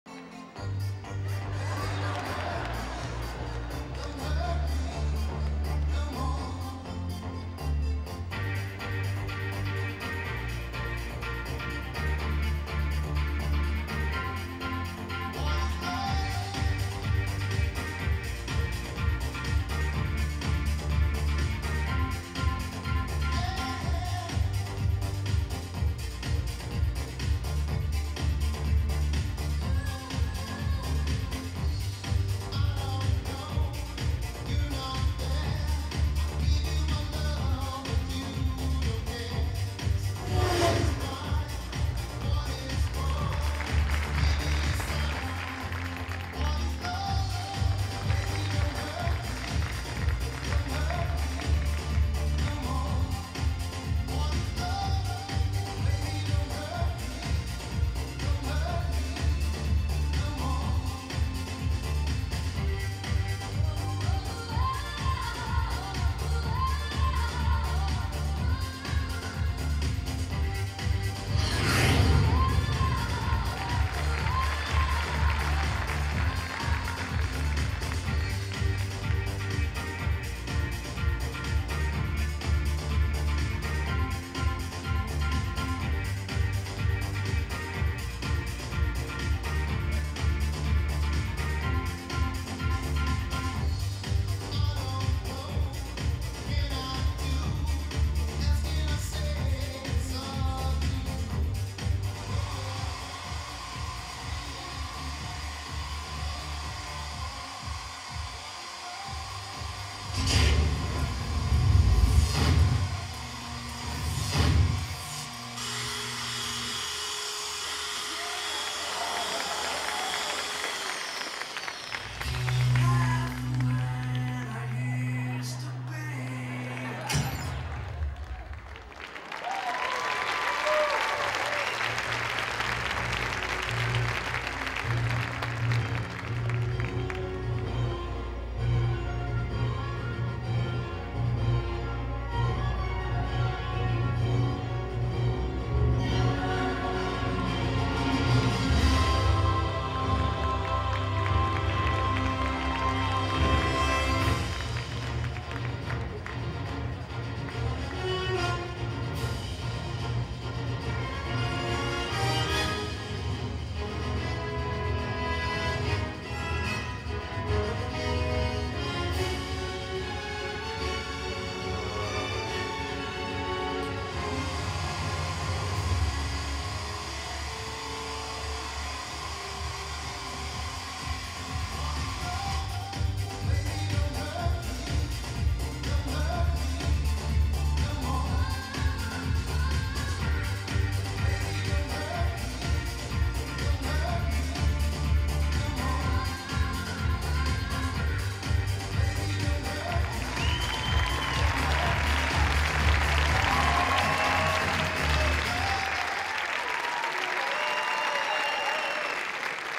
Jay Mattioli Hair Salon
Jay Mattioli performing his hair salon cutting in half illusion, live!